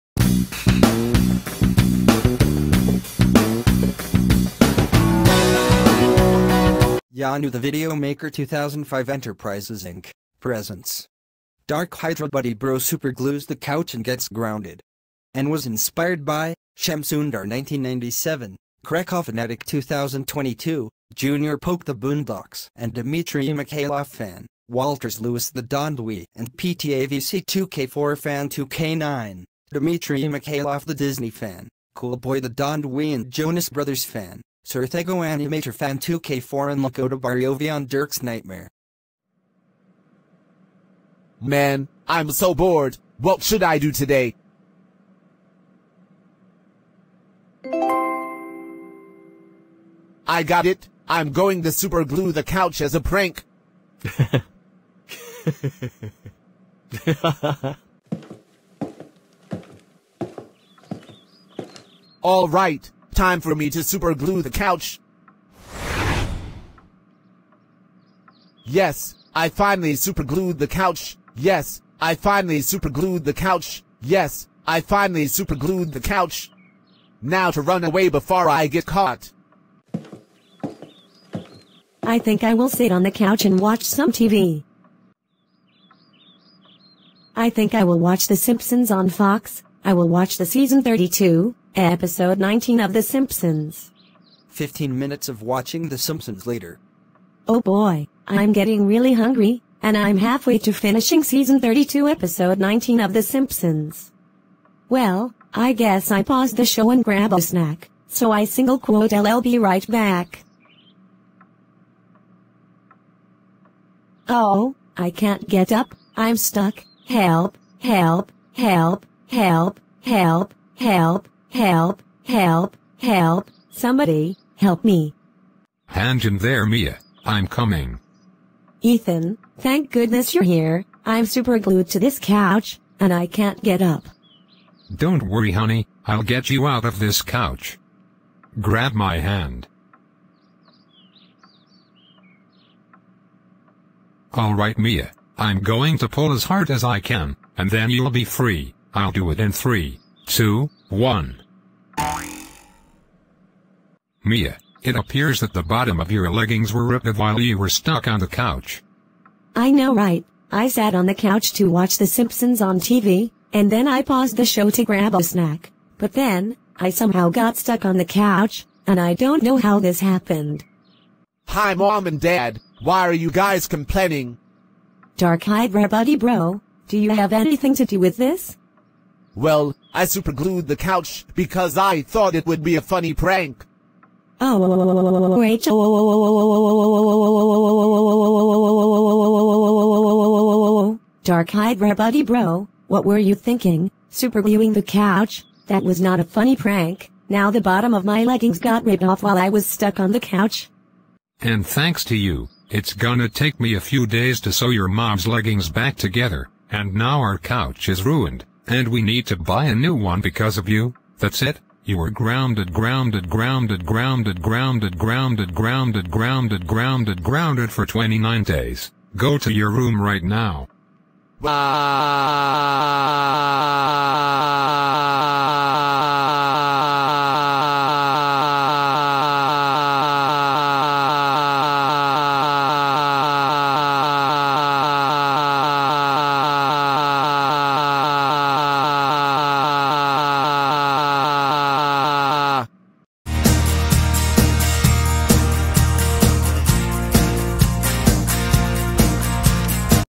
Dark Hydra Buddy Bro superglues the couch and gets grounded
Inspired by:@StarHunter2K4 ,@shamsundar1997 ,@juniorpolktheplotagonmovie4833 ,@walterlewisthedondwiiannoy7852 ,@DimitriyMykhaylovDisneyFan2006 ,@cool_mations ,@Surrey The Aubrey Cox & The Backyardigans Fan  and @dalococider @ericthevideoeditor2k8